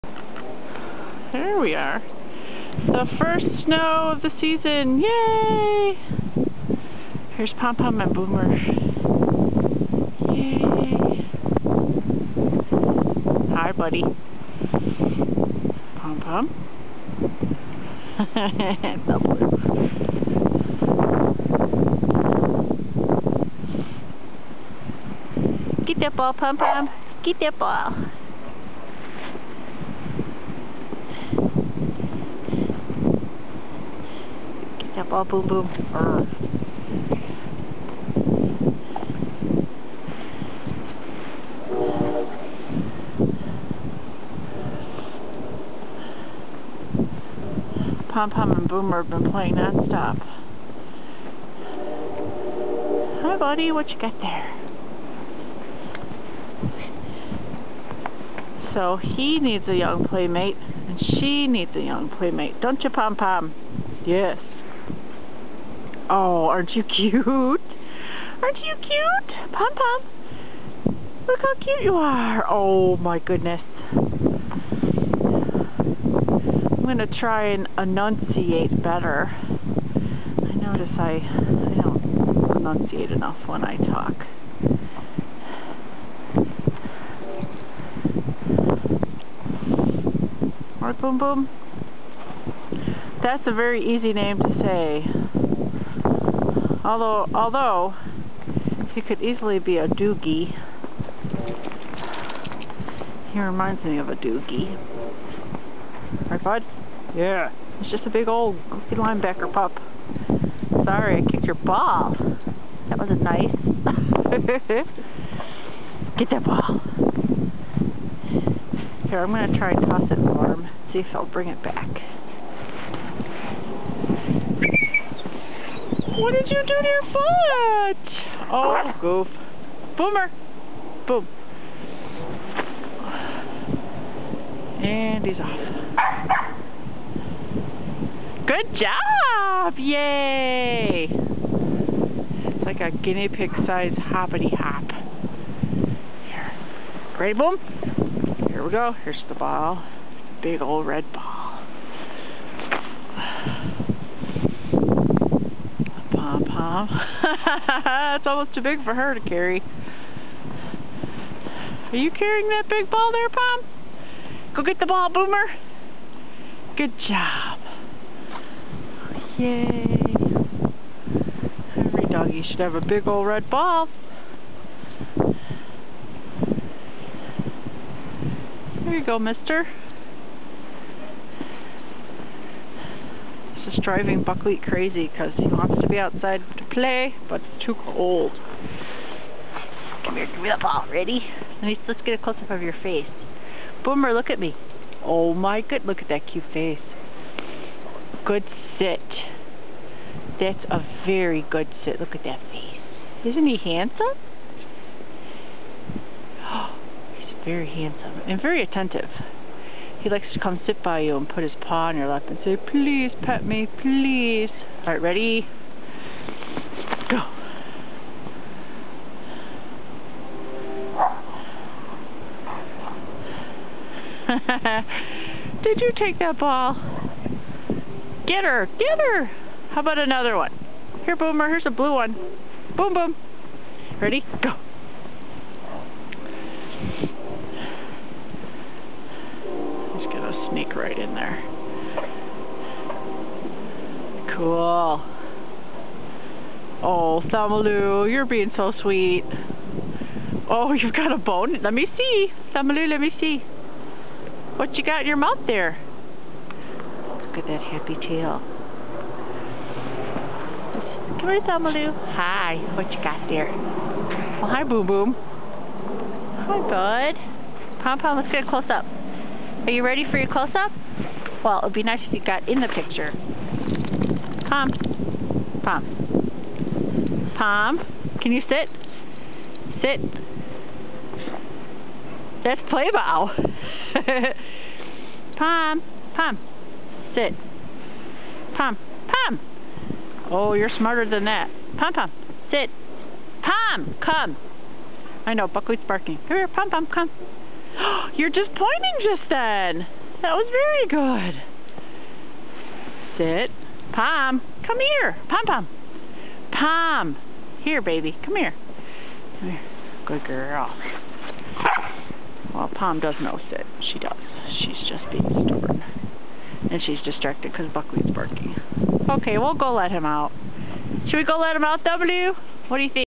The Big Red Ball.MOV
The first Chicago snowfall!!  Boomer and Pomegranate (Pointer and Setter mixes) take advantage of playing fetch in the snow.  Thelmalou (Setter) enjoys the snow too!